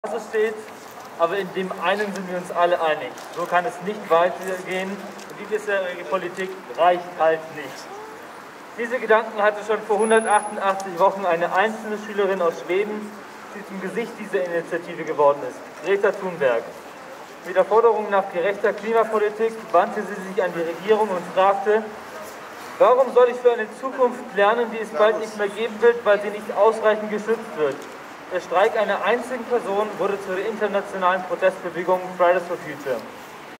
[2160p30] Fridays-For-Future-Demonstration in Hof (Saale) am 25.03.2022: Beginn der Bewegung
In diesem Videoabschnitt wird über den Beginn der Fridays-For-Future-Bewegung geredet.